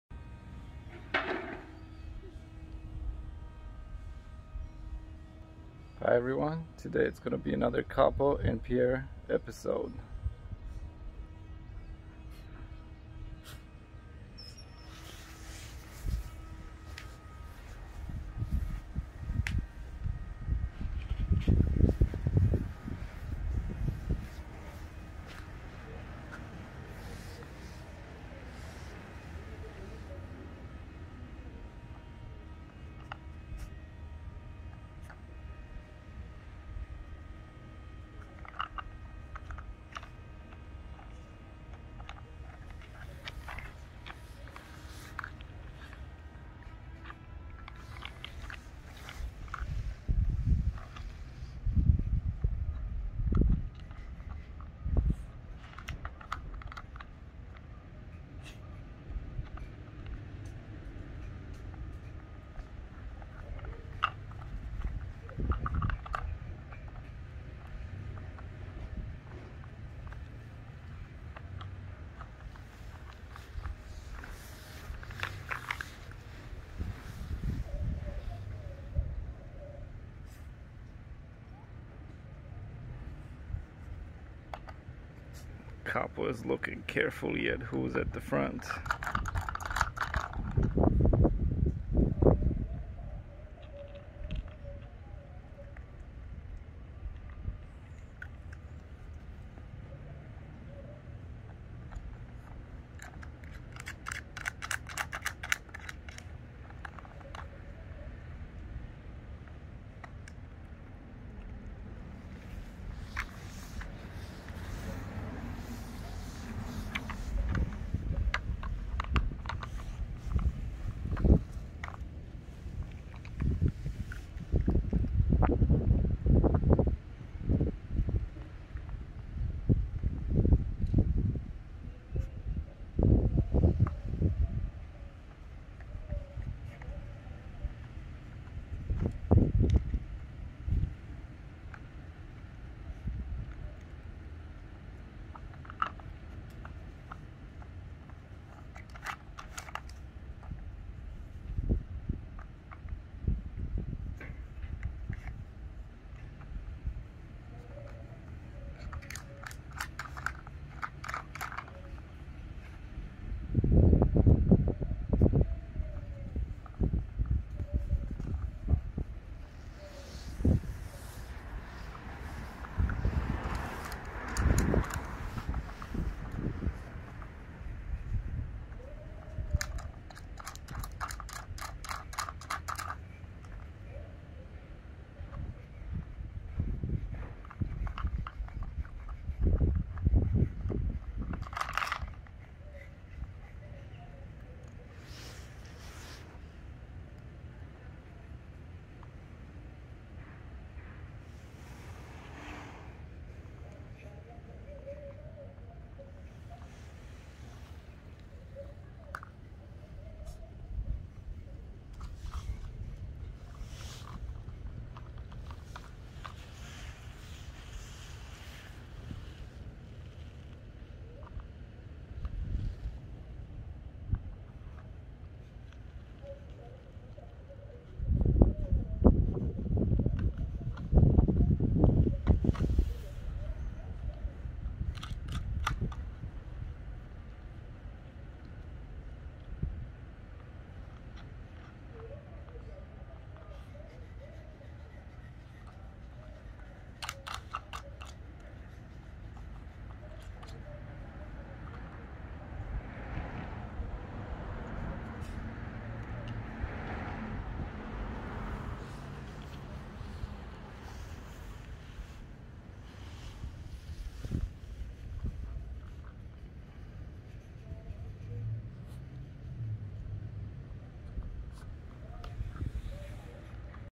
Very cute and sweet Bichon Frise brothers Capo and Pierre are back. 귀여은 비숑 프리제 강아지 개. Slatki Bišoni.
Capo and Pierre are back again. Once again Capo is more focused on guarding the house gate while Pierre plays the game to get some snacks. 귀여은 비숑 프리제 강아지 개. Slatki Bišoni se igraju i čuvaju kuću.